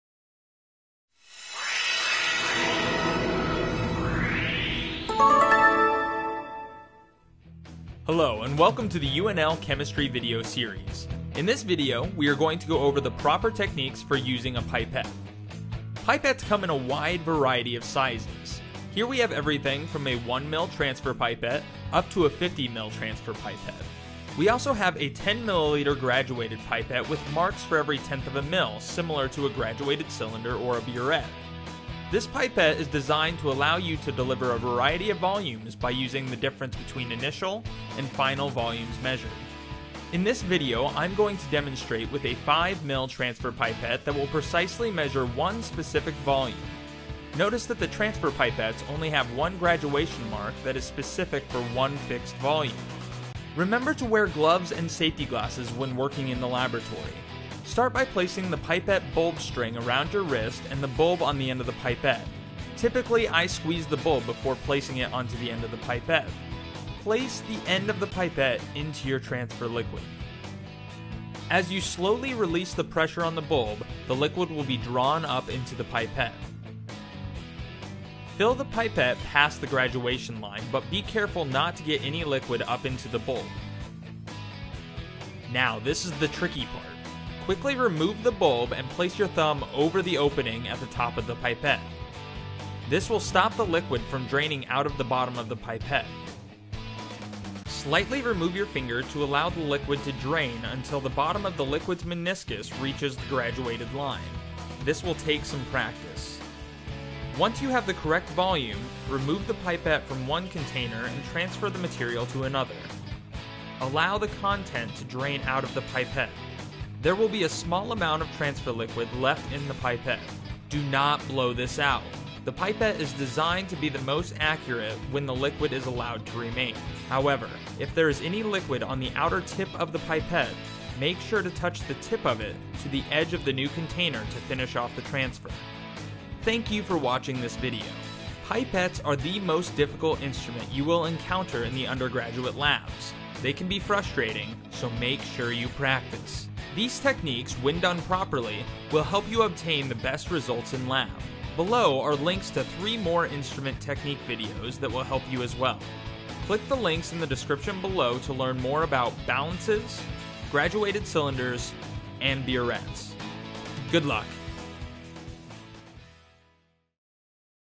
Pipet Technique
This video shows the proper technique for using a pipet in the
undergraduate chemistry laboratory.